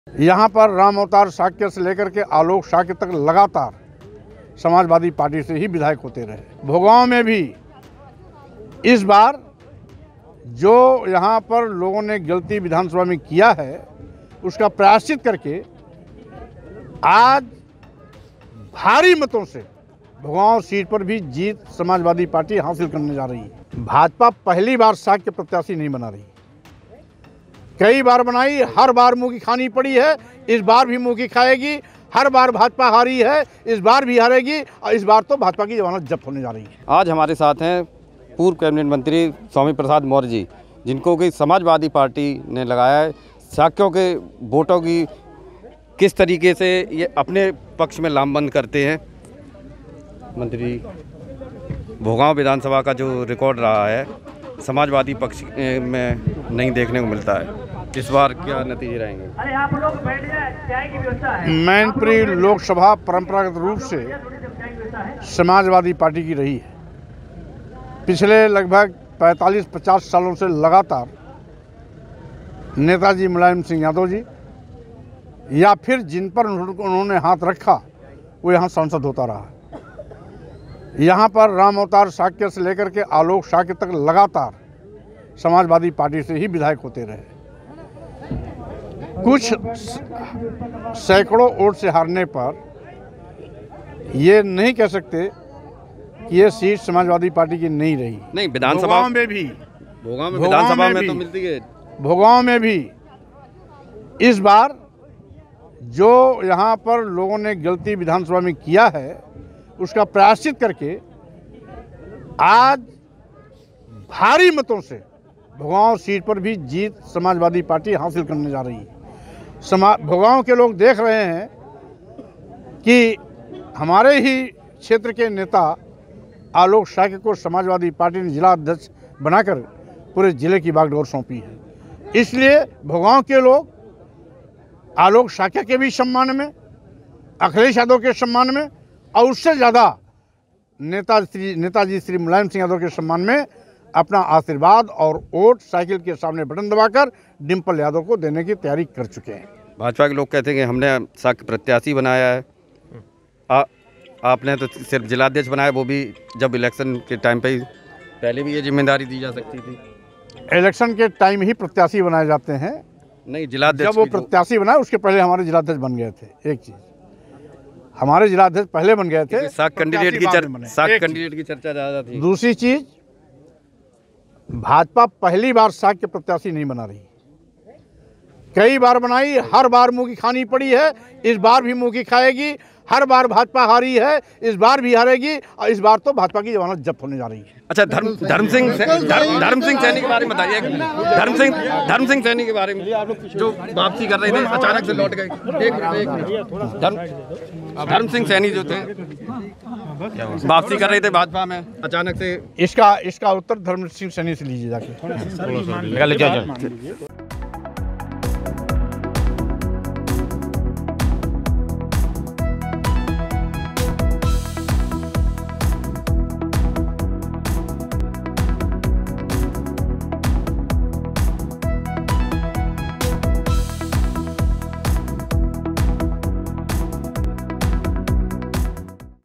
Mainpuri By Election: स्वामी प्रसाद मौर्य ने अखिलेश यादव का ये प्लान बता ही दिया...
स्वामी प्रसाद मौर्य ने मैनपुरी उपचुनाव को लेकर कही बड़ी बात. UP Tak से खास बातचीत में अखिलेश यादव का बड़ा प्लान बता दिया..